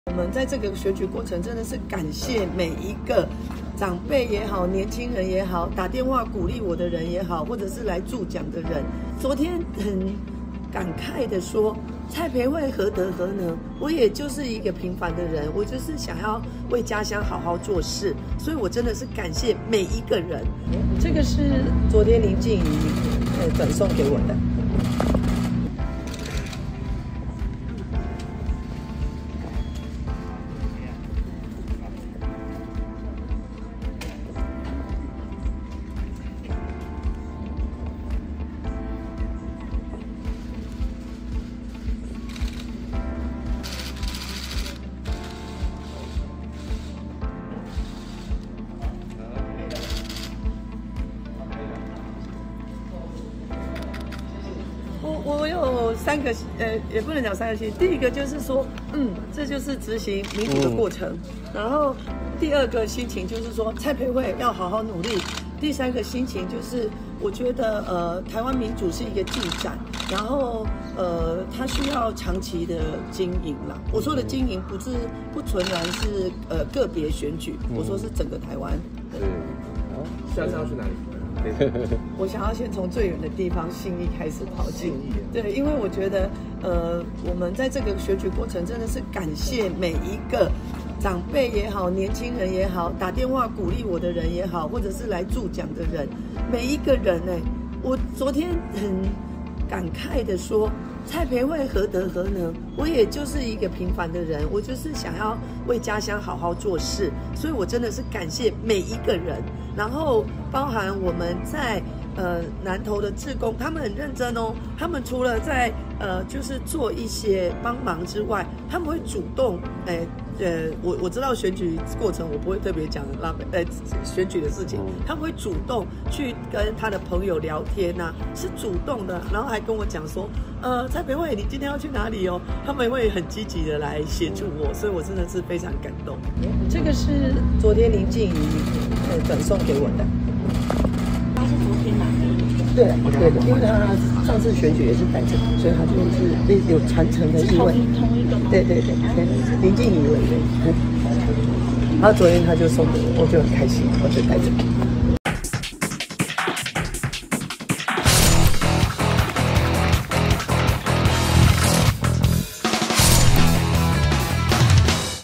南投立委補選！蔡培慧秀幸運小物 「老天爺會肯定認真的人」│@politics_setn
友邦連結區
◎ 訂閱【三立新聞網】最新影片搶先看